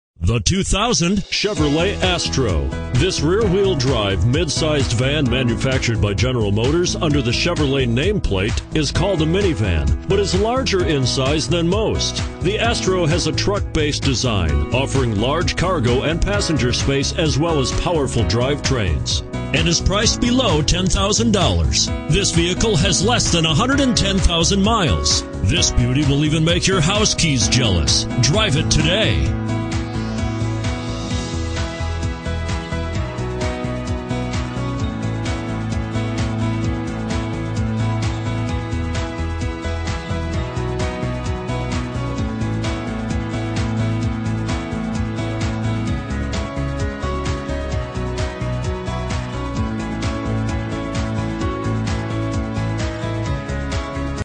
2000 CHEVROLET ASTRO PASSENGER White Bear Lake, MN 20516
2000 CHEVROLET ASTRO PASSENGER White Bear Lake, MN
Stock# 20516

1801 E Co Road F

Want to stretch your purchasing power? Well take a look at this good-looking 2000 Chevrolet Astro. Well Maintained. It is nicely equipped with features such as AWD, 3rd row seats: bench, ABS brakes, Air conditioning, AM/FM radio, Front Bucket Seats, Power Windows, Rear Heater, Remote Keyless Entry, and Remote Panic Alarm. You never know when life is going to throw you a curve, but the antilock braking system will always be there to help stop trouble right in its tracks. Polar is the safest, easiest place on earth to buy a vehicle...no kidding! Call, e-mail, or stop by today!